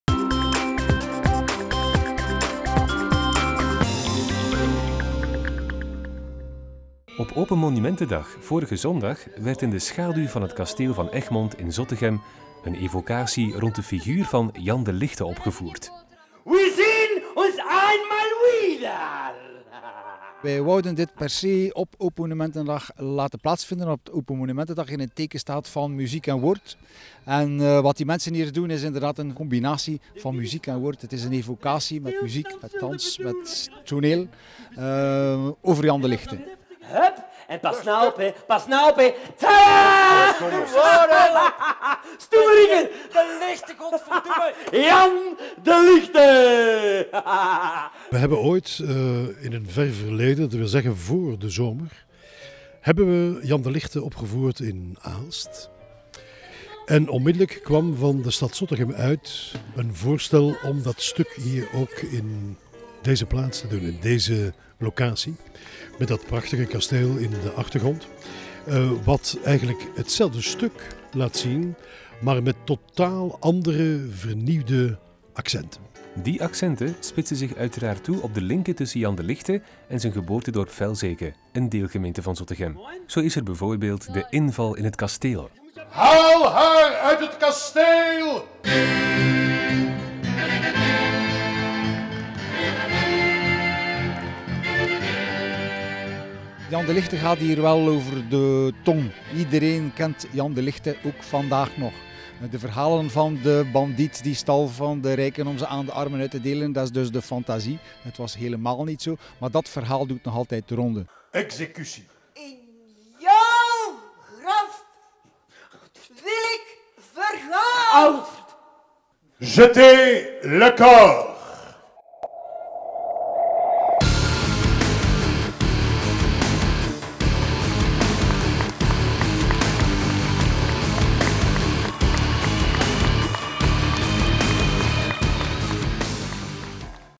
StadsTV Zottegem - Evocatie Jan De Lichte
Tijdens de Open Monumentdag werd in het park van het Kasteel van Egmont het verhaal rond Jan De Lichte opgevoerd.  O.a. de vorming van de bende van Jan De Lichte, de inval in het kasteel en de terechtstelling kwamen aan bod.  De regie was in handen van Anton Cogen.